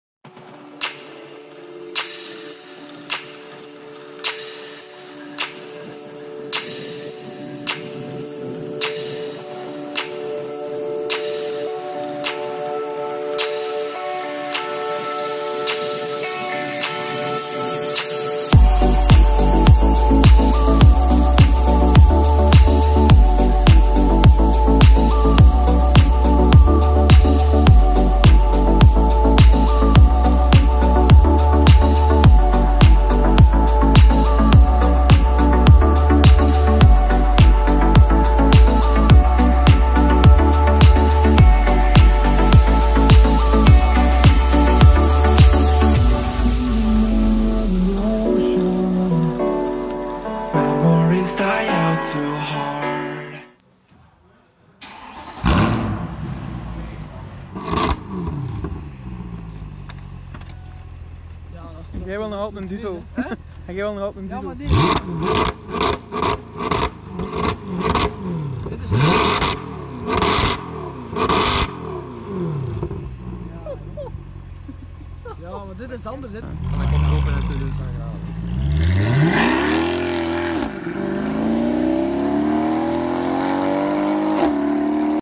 STRAIGHTPIPING A C63 AMG ESTATE - SUPERSPRINT X PIPE - ICON AUTOMOTIVE (LOUD! / SOUNDCLIP)
Video of ICON AUTOMOTIVE straight-piping a C63 AMG estate

Supersprint X-pipe

SEND US IF YOU WANT MORE INFO !